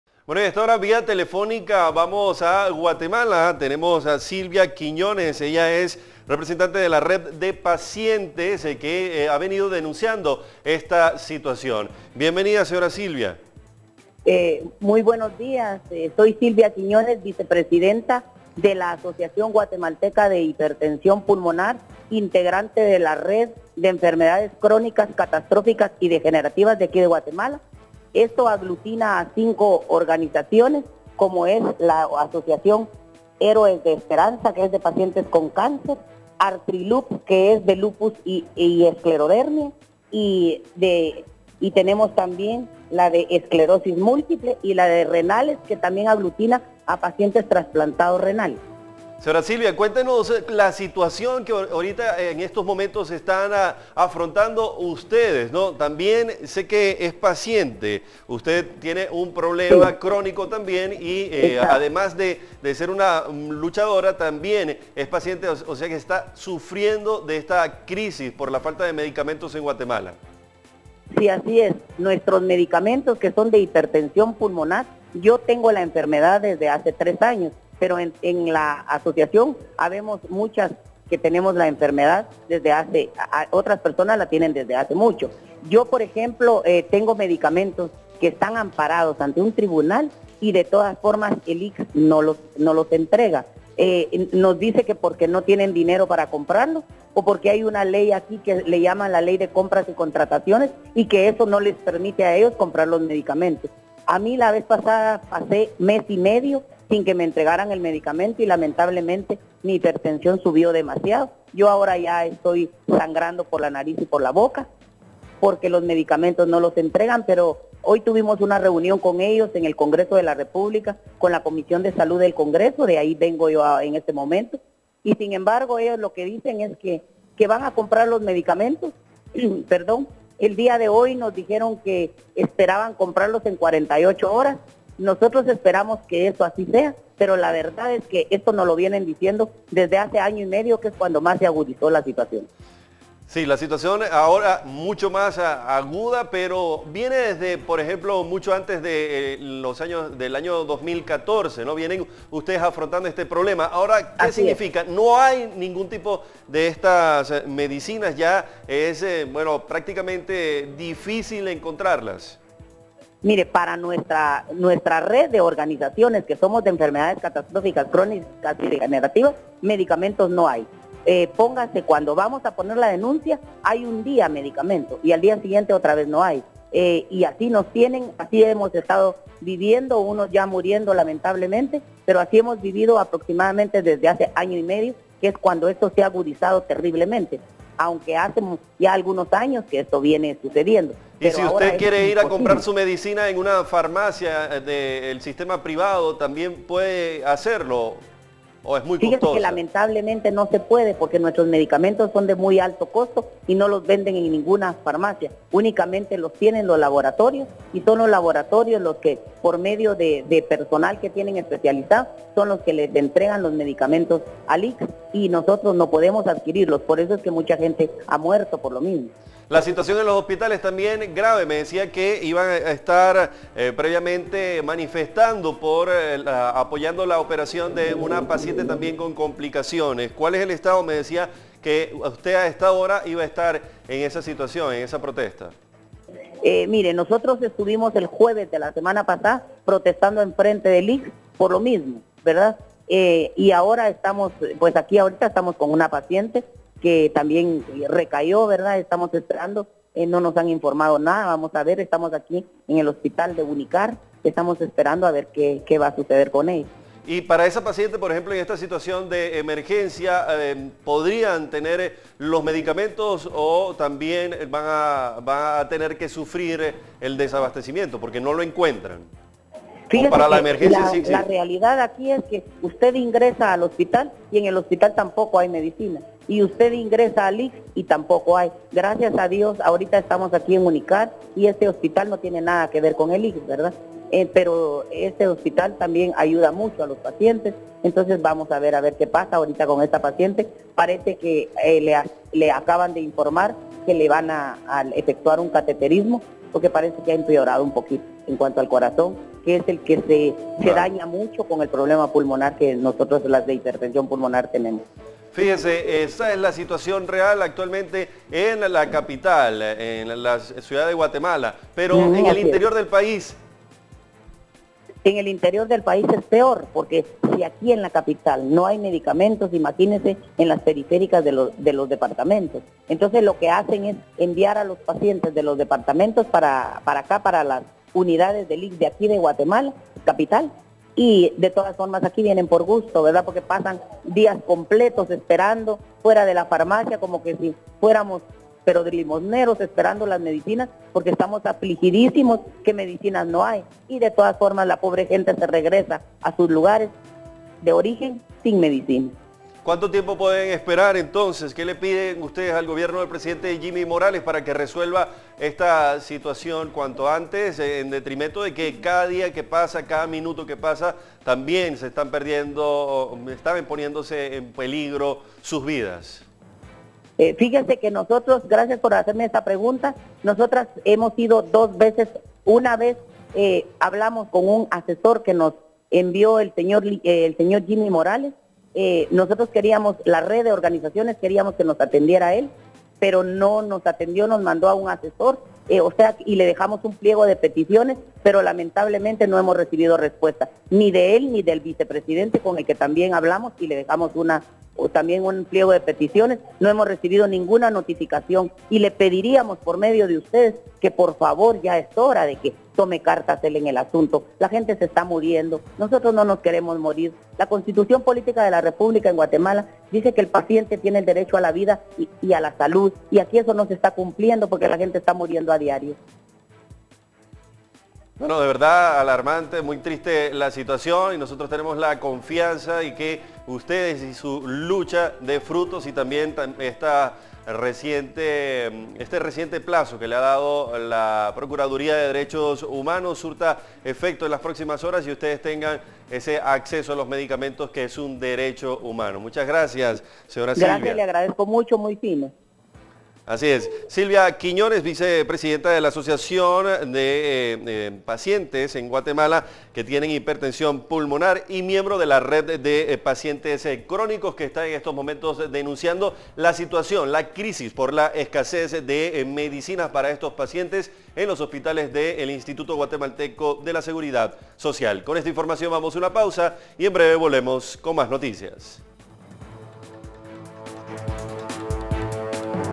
Pacientes crónicos en peligro de muerte por falta de medicamentos en Guatemala
Un viacrucis viven miles de enfermos guatemaltecos que no pueden llegar a curarse, porque en los hospitales no hay las medicinas que necesitan para sobrevivir.

Silvia Quiñonez, vicepresidenta de la Asociación Guatemalteca de Hipertensión Pulmonar, es también miembro de la Red de Pacientes, un grupo de organizaciones que luchan por el derecho a la salud de los enfermos crónicos, en comunicación con CB24 Noticias de Centroamérica, reveló el sufrimiento de enfermos y familiares que ven deteriorada su calidad de vida al no poder cumplir con sus tratamientos.

Silvia Quiñonez también es paciente y cuenta como por no tener sus medicamentos ahora sufre complicaciones y su enfermedad está empeorando. Como ella cientos de guatemaltecos no consigue parar el dolor y pasan horas a las puertas de las farmacias de los hospitales del Instituto Guatemalteco de Seguridad Social,   esperando recibir su receta y en el mejor de los casos les entregan productos genéricos que no, según ello no les surten los mismos efectos.

Quiñonez confirmó que se han comunicado con el presidente Jimmy Morales pero no han sido atendidos y ni han recibido respuesta.

Los pacientes confían que el ultimátum que le dio la Procuraduría de Derechos Humanos de Guatemala, para que las autoridades atiendan el caso, traiga una solución efectiva y permanente a la crisis de escasez de medicinas.